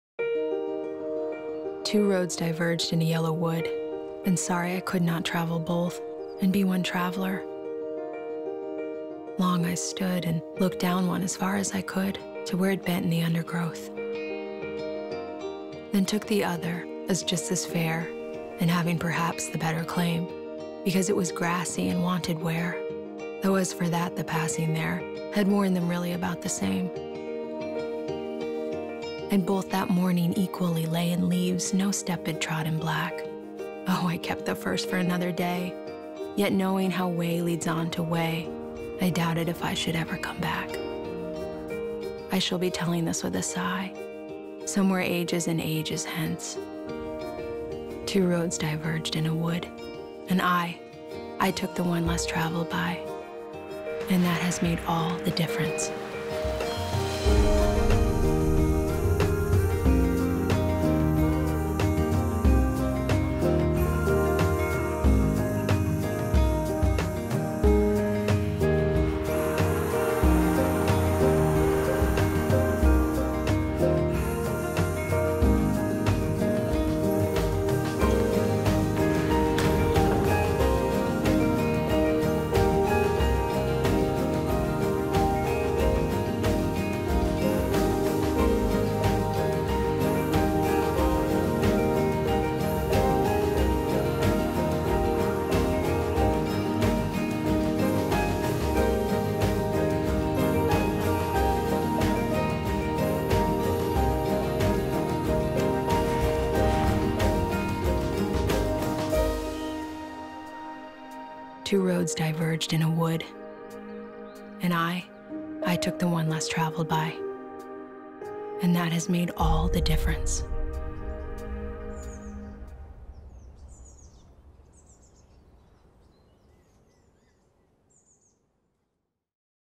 Find Your 'Road Less Traveled' at IUP
In this video, Robert Frost's poem "The Road Not Taken" takes the viewer on an introspective tour of the Indiana University of Pennsylvania (IUP) campus. IUP invites you to find your "road less traveled." Let the Oak Grove at the center of campus be your "yellow wood" as you choose from over 130 majors to find the perfect path to your career success.